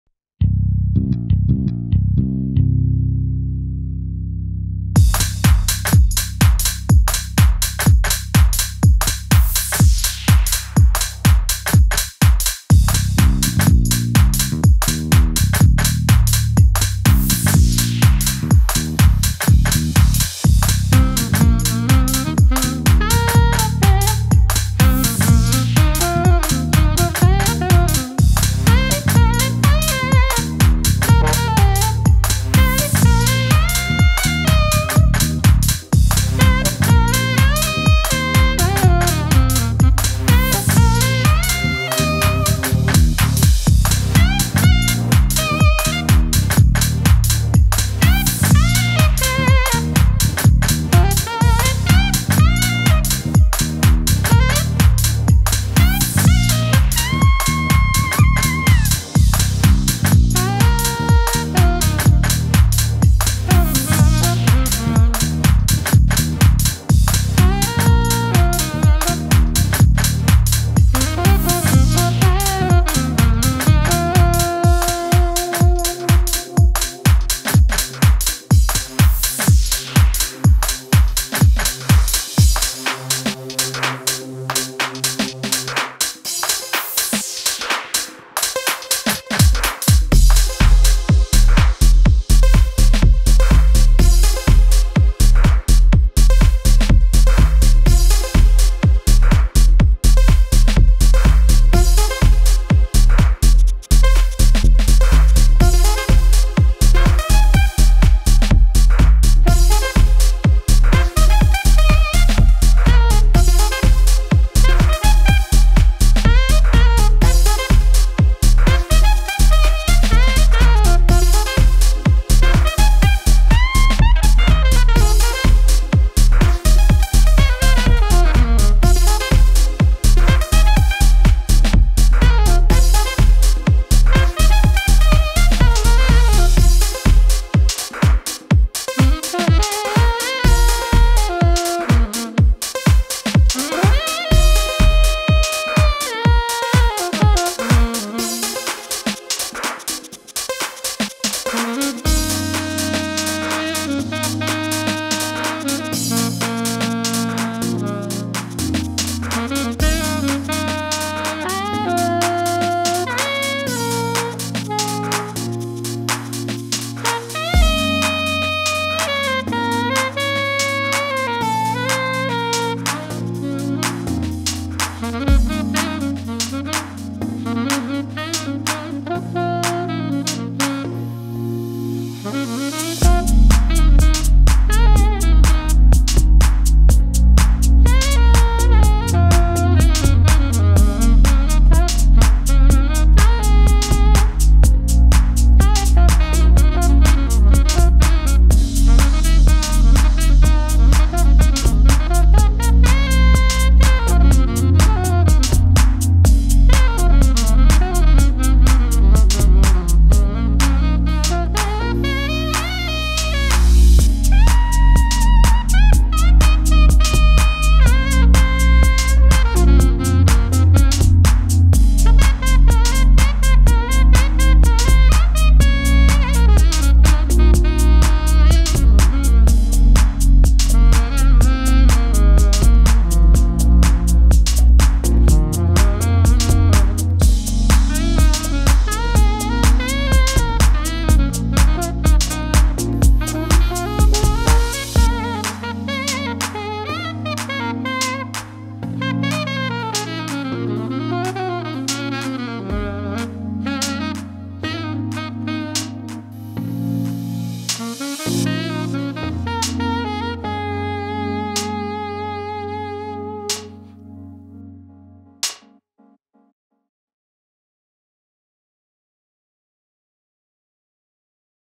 NNP Sessions: Sax in isolation
A short "distance" jam we did with the awesome Ivan Blanusa on sax.. trying to get creative and have some fun during this entire "human malware" situation.
Stay safe, have fun and groove on!

Video recorded and edited by MDM Visuals